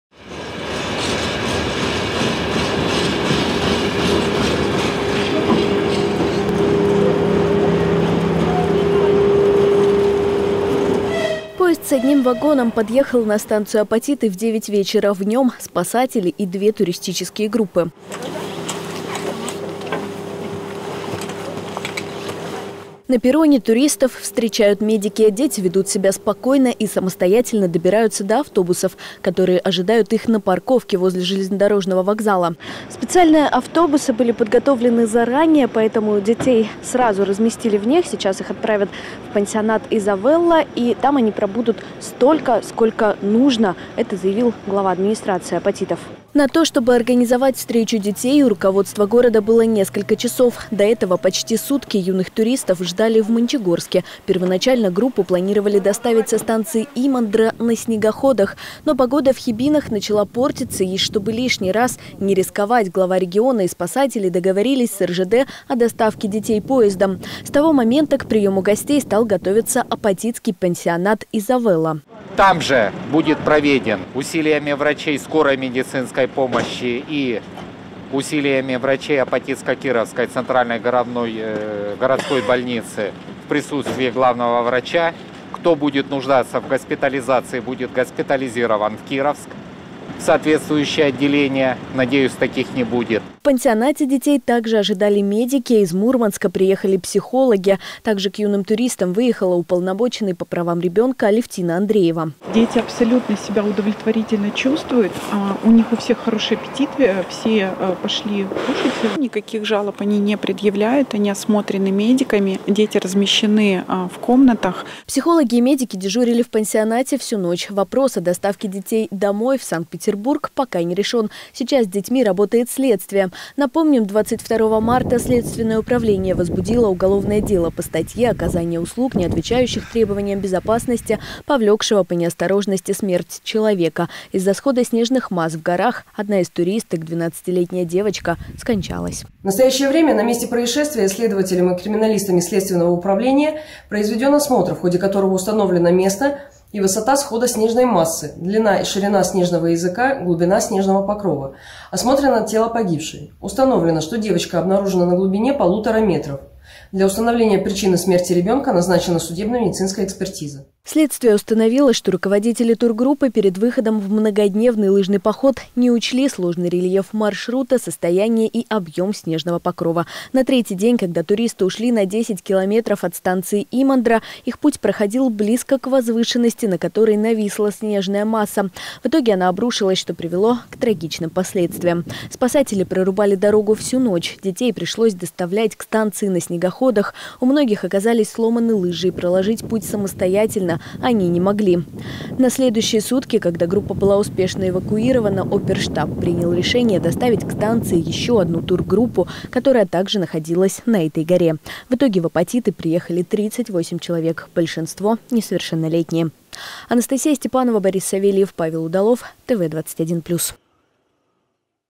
Пострадавшие в Хибинах туристы временно живут в Апатитах
Над спасательной операцией работали 84 человека и 33 единицы техники.